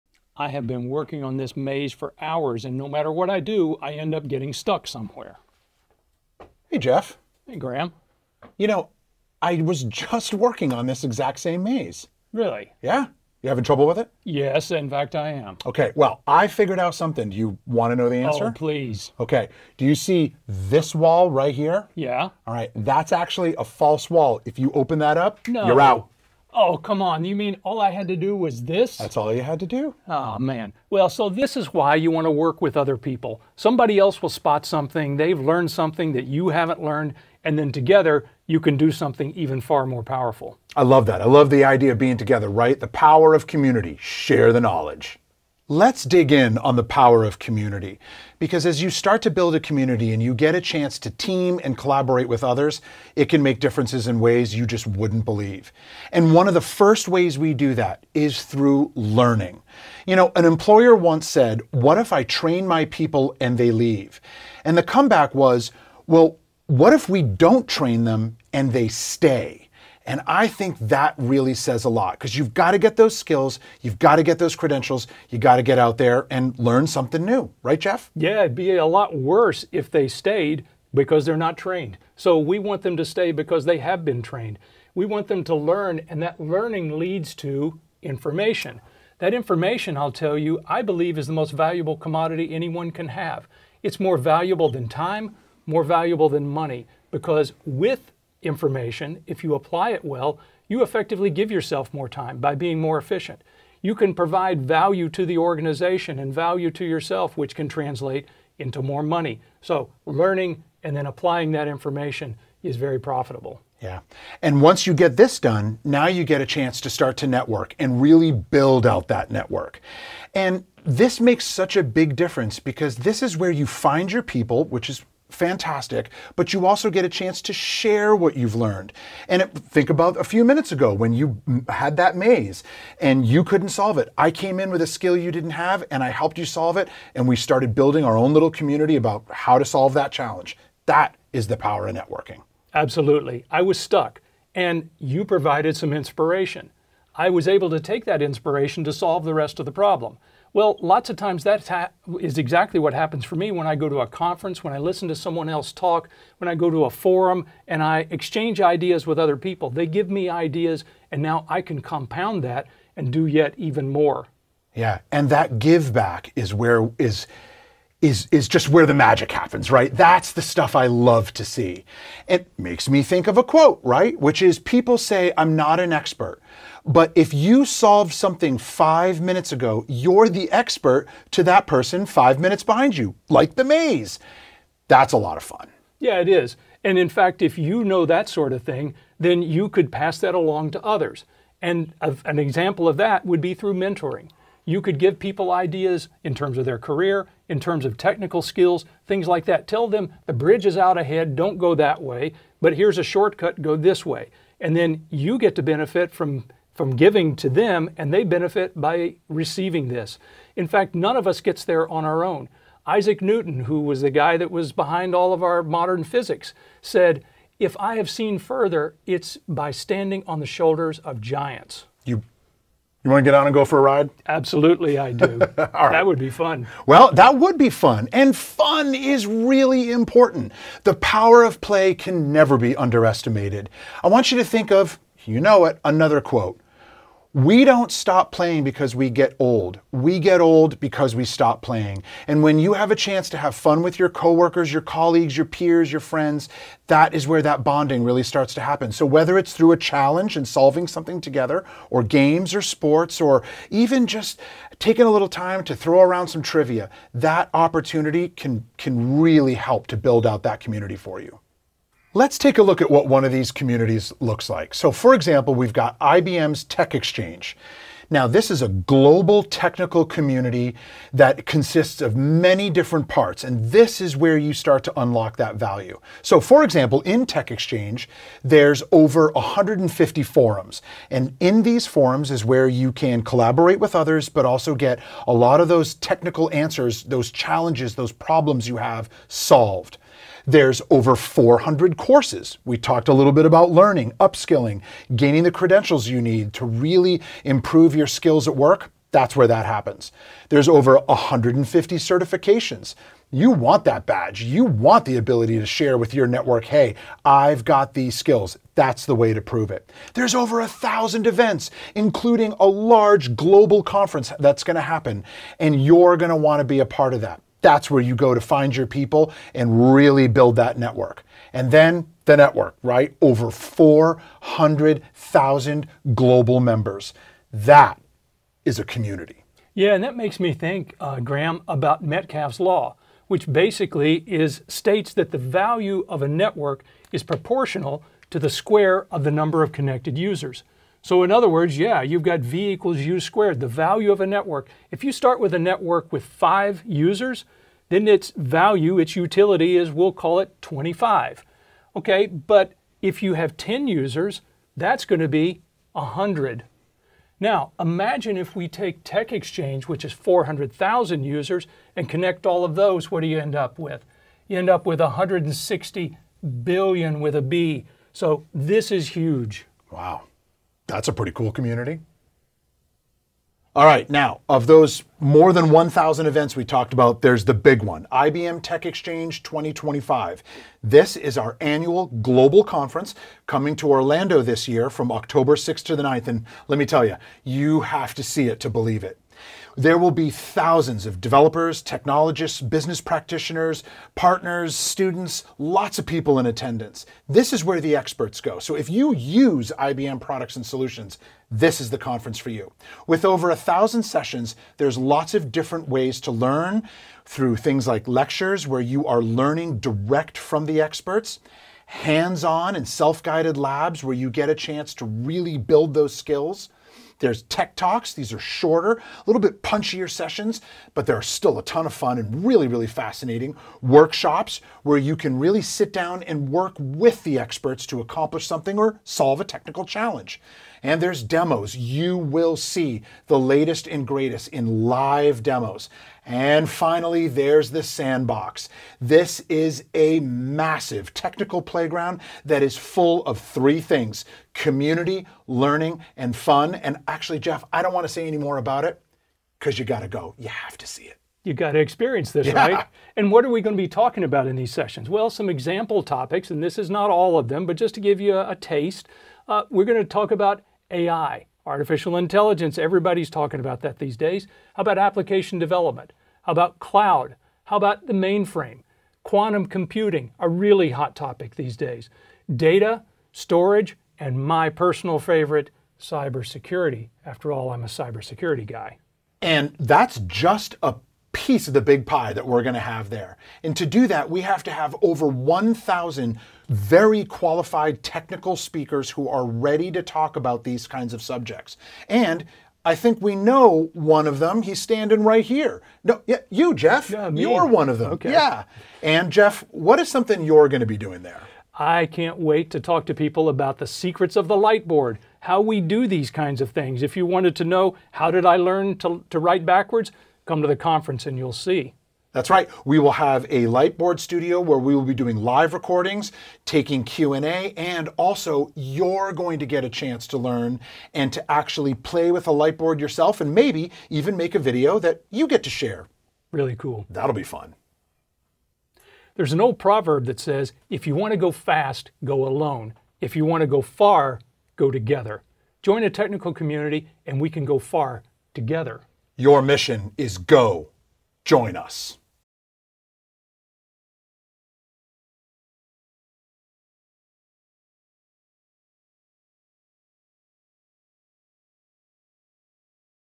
The Value of Technical Communities: Learn, Share, & Grow Together
🤝 Stuck on a problem? Jeff Crume and Graeme Noseworthy reveal the value of technical communities! Learn how collaboration, networking, and upskilling can solve challenges, boost your career, and unlock innovation. Explore how sharing knowledge and building connections creates growth opportunities. 🚀

AI news moves fast. Sign up for a monthly newsletter for AI updates from IBM